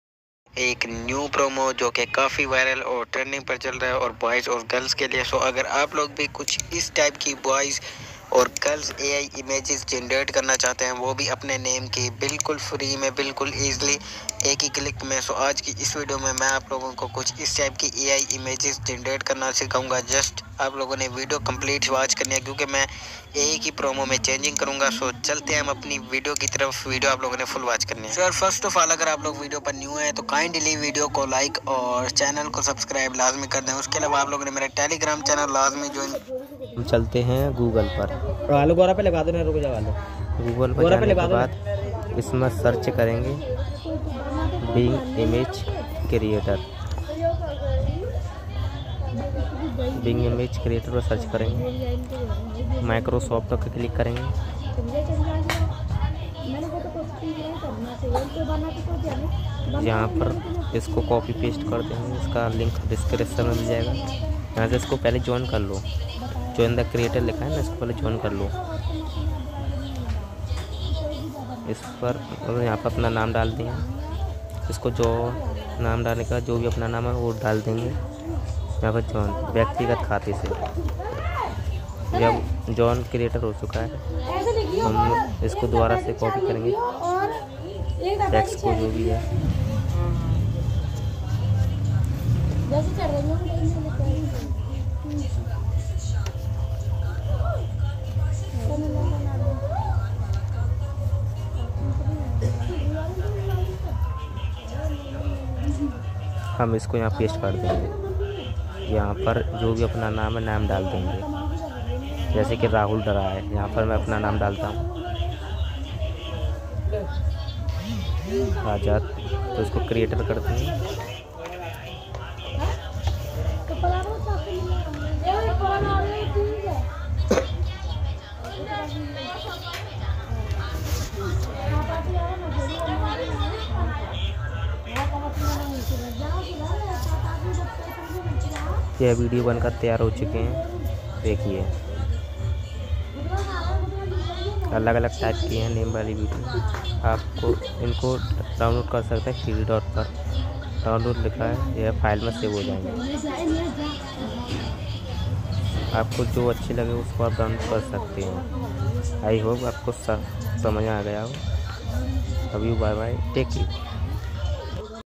Rich style Attitude boy photo editing/ bing app photo editing/ new ai photo editing bing ai
Promo 👇👇⬇️⬇️

For Boys 🔥

Create a 3D illustration featuring a realistic 20year-old beautiful young boy with black Hoodie and black Jeens, white iPhone holding in hand on a Wingback chair, wearing sneakers, beautiful hair, luxury glasses, he looks ahead. Golden ROLLSROYCE Car in his Side, with name "RAHUL" written on big font on hoodie and Number plate, 3d art painting realistic photo with two security guard in my background With security dressing, on Dubai

For Girls 💞

Create a 3D illustration featuring a realistic 20year-old beautiful young girl with Garnet Hoodie and black Jeens, white iPhone holding in hand on a Wingback chair, wearing sneakers, beautiful hair, luxury glasses, he looks ahead. Golden ROLLSROYCE Car in his Side, with name "AISHA" written on big font on hoodie and Number plate, 3d art painting realistic photo with two security guard in my background With security dressing, on Dubai

Promo 👇👇⬇️⬇️

For Boys 🔥

For Girls 💞

Promo 👇👇⬇️⬇️

For Boys 🔥

For Girls 💞